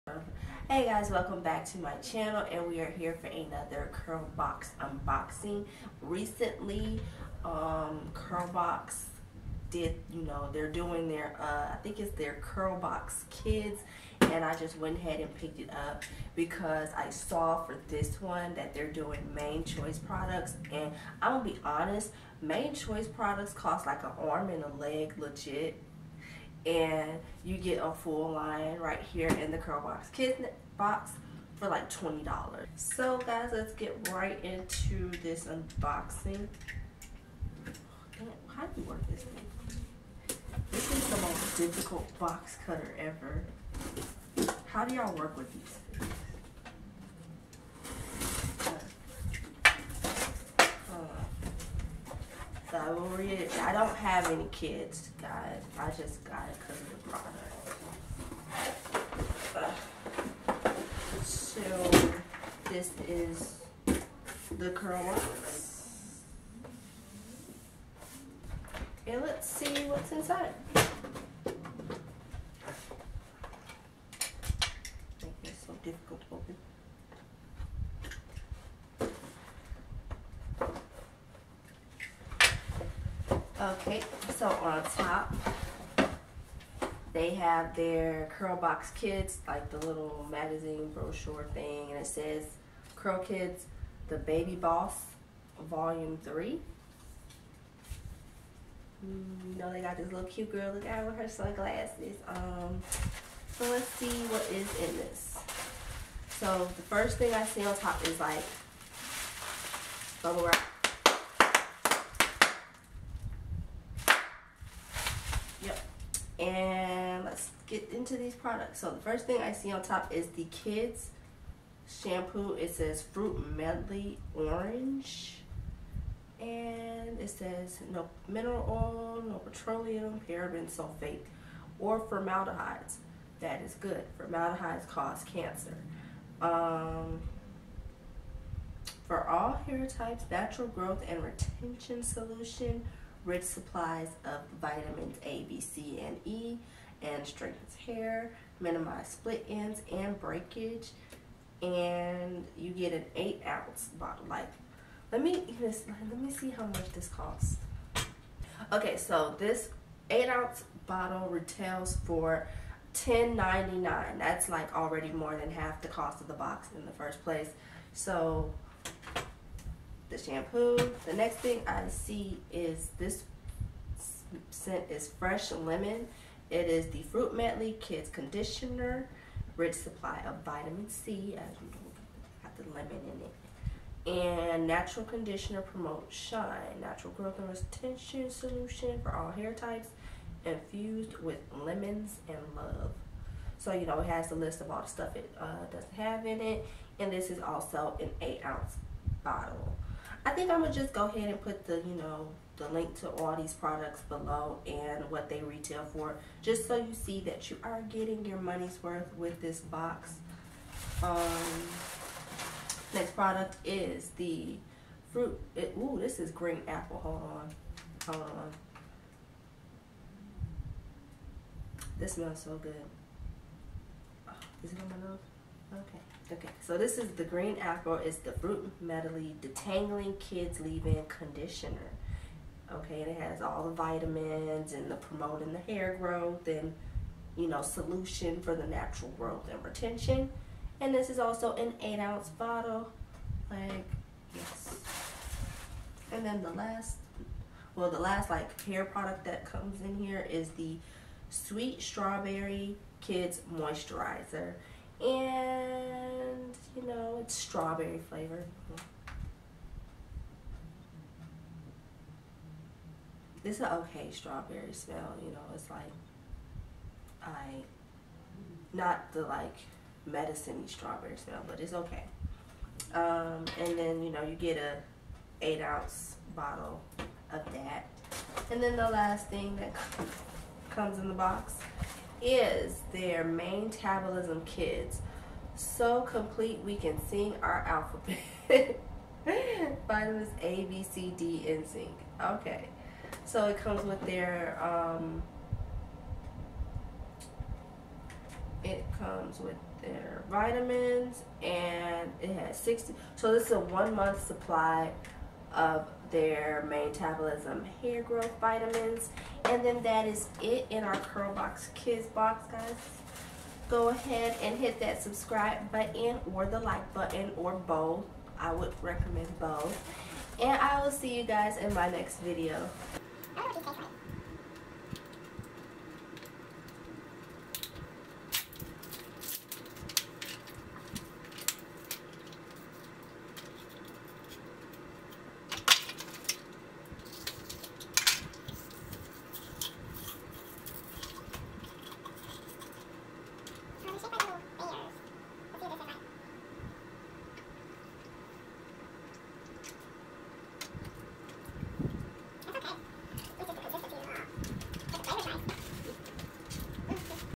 CURLBOX KIDS JANUARY 2017
Watch me unbox the curl box kids for the month of january